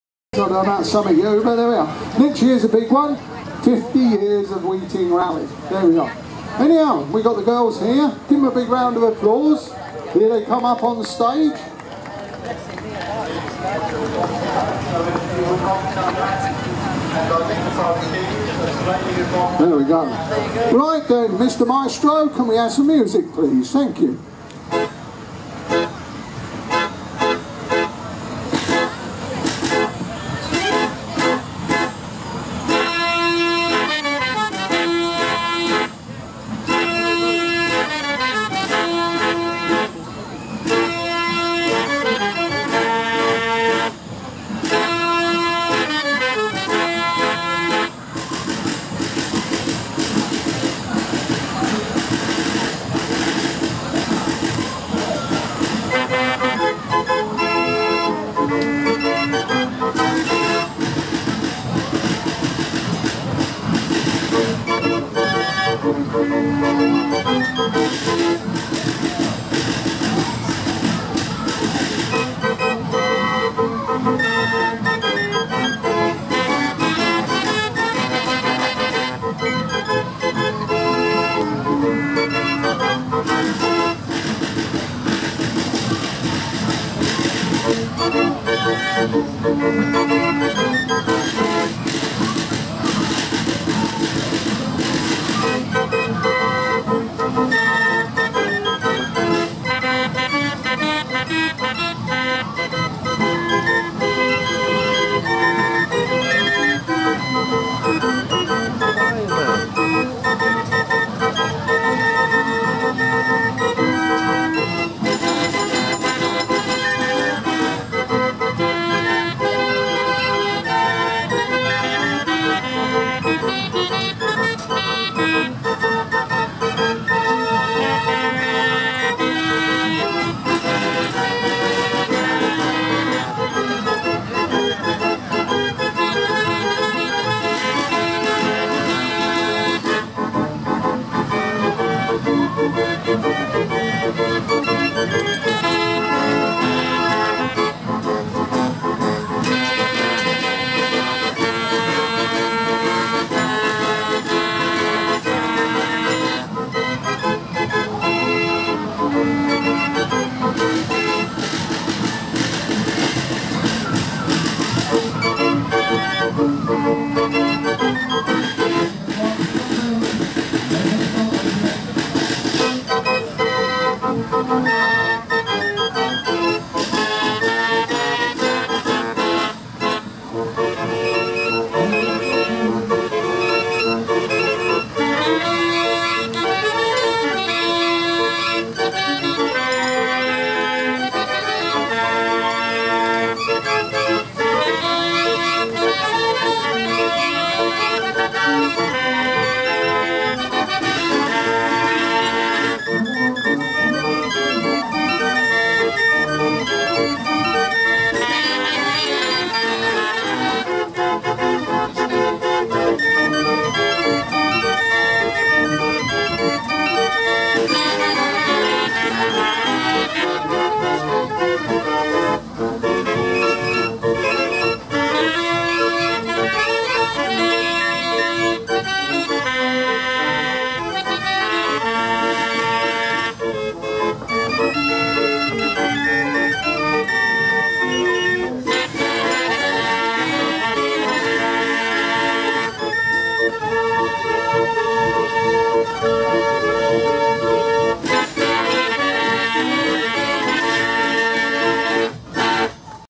The Girls return, dancing to the music of the wonderful old organ :)
Weeting Steam Engine Rally 2017